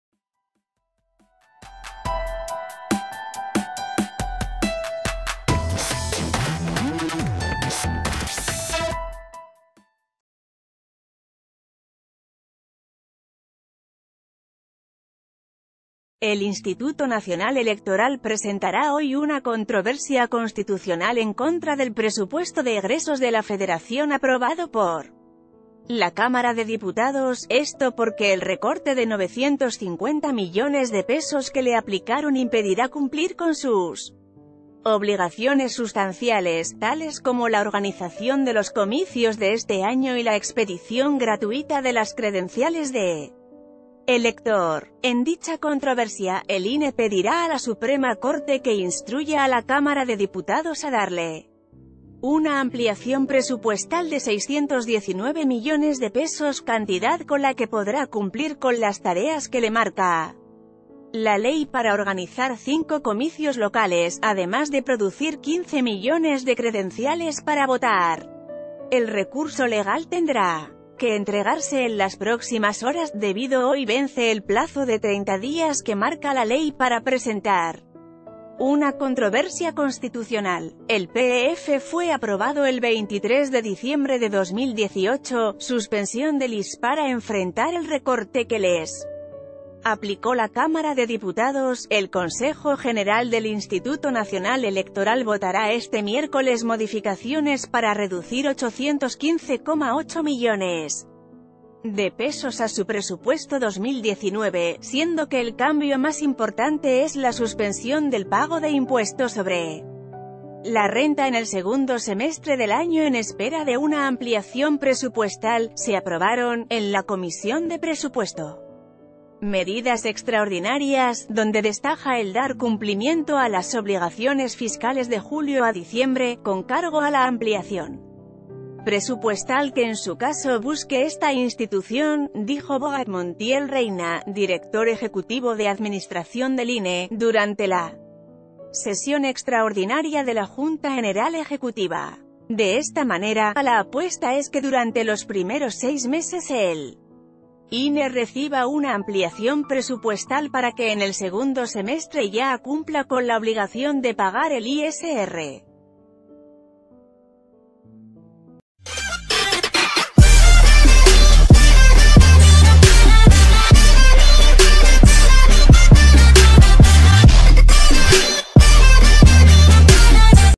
INE alista controversia ante Suprema Corte contra recorte presupuestal
El Instituto Nacional Electoral presentará hoy una controversia constitucional en contra del Presupuesto de Egresos de la Federación aprobado por la Cámara de Diputados, esto porque el recorte de 950 millones de pesos que le aplicaron impedirá cumplir con sus obligaciones sustanciales, tales como la...